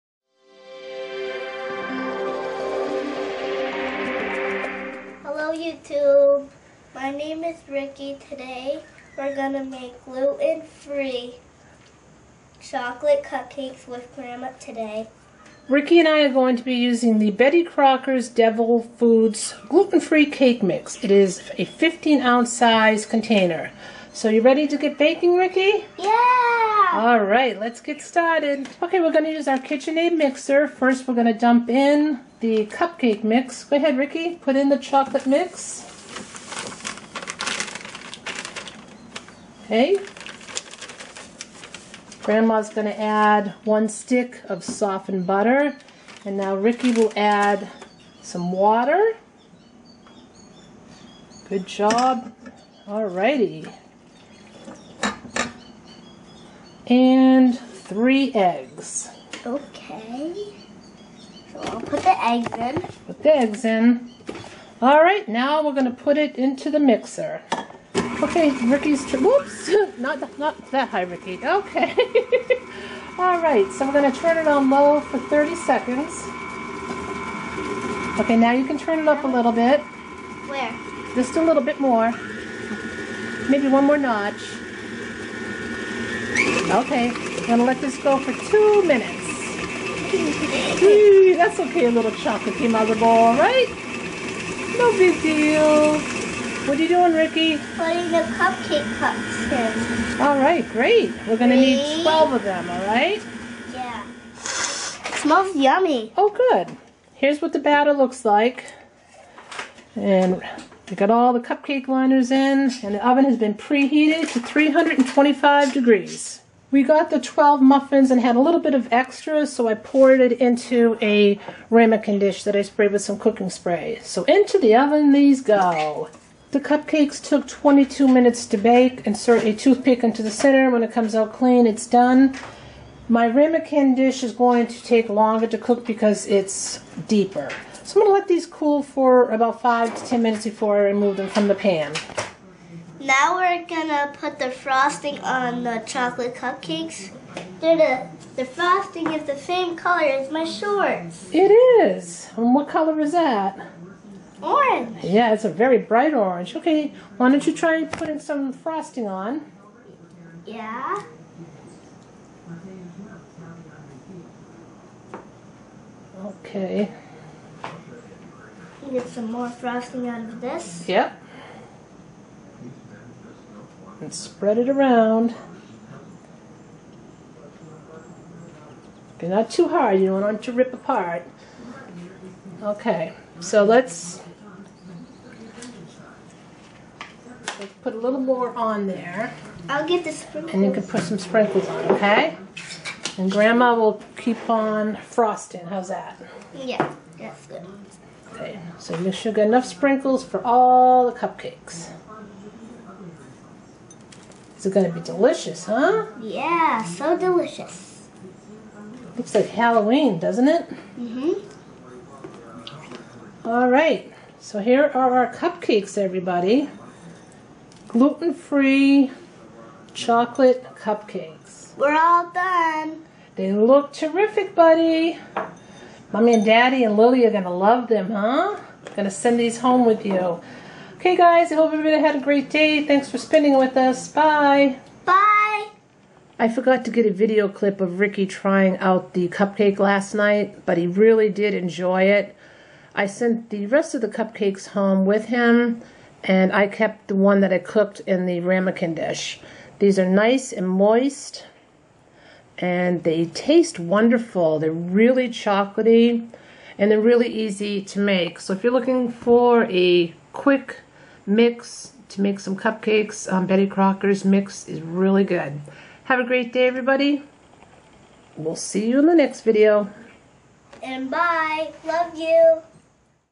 Gluten Free Devil's Food Cupcakes ~ Baking with Ricky
This was the first time I made the Betty Crocker Devil's Food Gluten Free Cake Mix. 15 OZ. Box.
My 5 year old Grandson Ricky helped me to bake the Cupcakes. He loves to bake.
These cupcakes are simple to make and have a great Chocolate taste and are moist.

I got 12 Cupcakes and 1 miniature loaf  Ramekin or you could have gotten 14 cupcakes.

I bought this mix at Walmart for $4.18

I always like to have a quick mix available for when my Grandson comes over. I will be buying this mix again.

The recipe is on the back of the box:

Heat oven to 350* for a shiny metal pan or 325* for dark or nonstick pan. Grease bottom only of the cake pan or use cupcake liners for muffins.

Beat in a Mixer:
Cake Mix
1 Cup Water
1 Stick Butter, softened
3 Eggs
Mix on low for 30 Seconds, then on medium speed 2 minutes. Scraping the bowl occasionally. Pour into Pan.

Bake until a toothpick inserted into the center comes out clean. Cool completely before frosting.

12-14 Cupcakes 18-23 Minutes
8X8 pan 44-49 Minutes
9X9 pan 38-43 Minutes
8" or 9" round pan 43-48 Minutes

Camera:
Canon PowerShot SX230 HS 14X Zoom

Editor:
Corel ~ Videostudio ProX4 Professional Video Editing Software